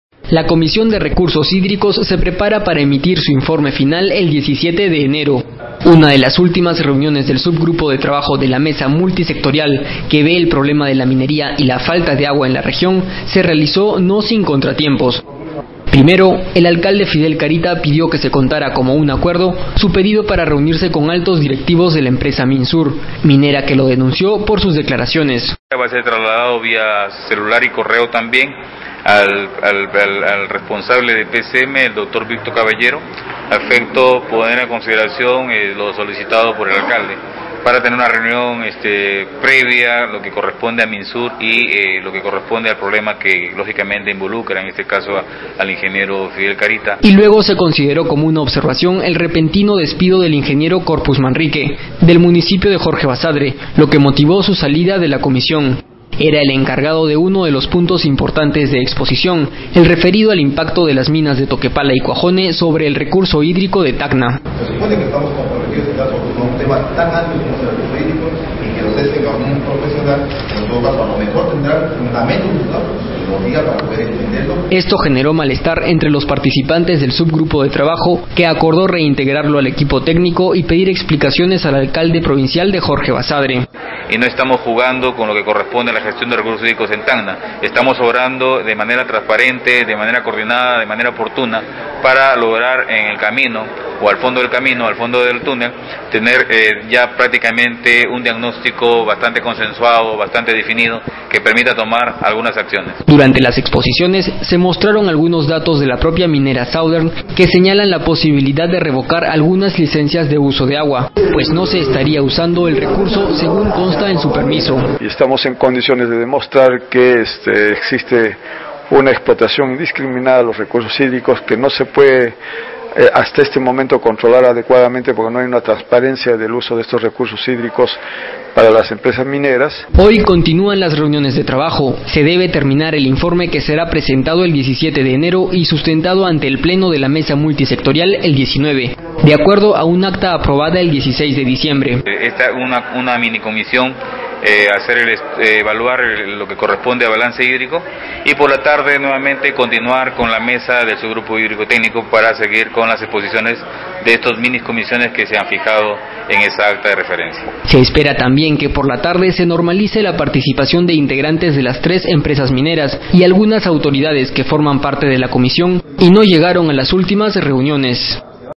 RECURSOS HÍDRICOS PRESENTA INFORME FINAL EL 17
La comisión de recursos hídricos se prepara para emitir su informe finalel 17 de enero.